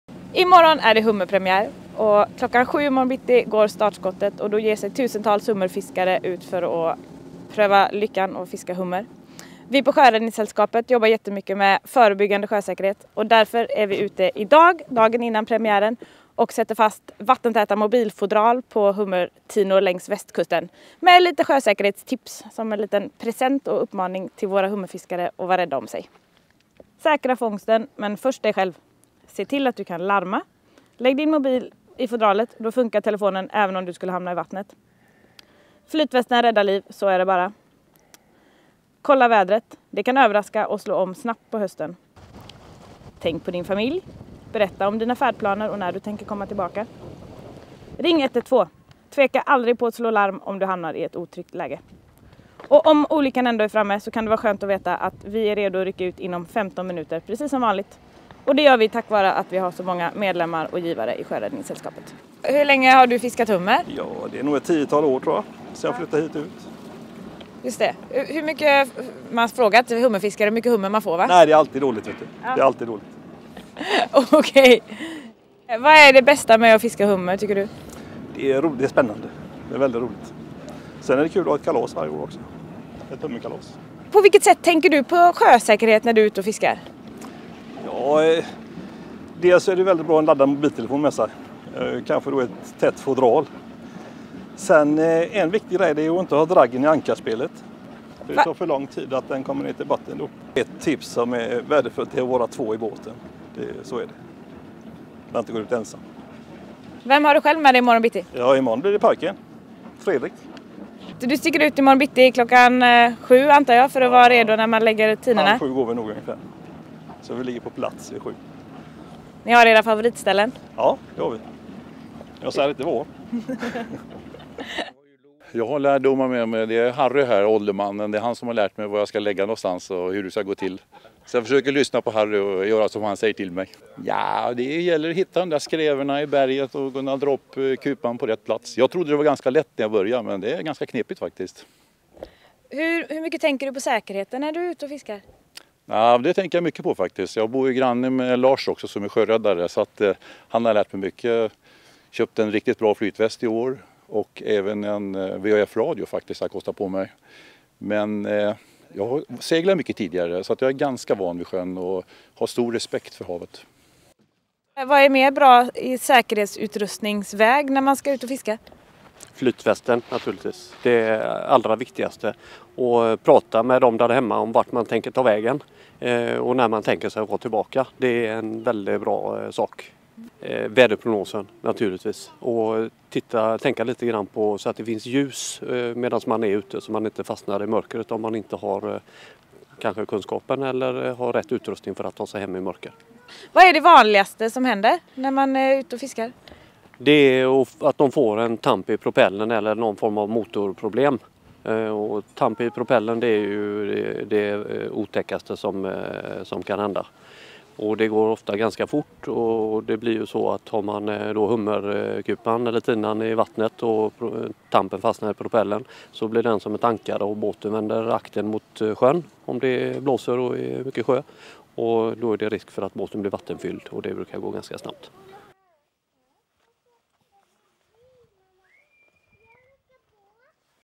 Sjöräddningssällskapet inför hummerfiskepremiären 2015 ny
Sjöräddningssällskapet delar ut vattentäta mobiltelefonfodral och informerar om säkerhet inför hummerfiskepremiären 2015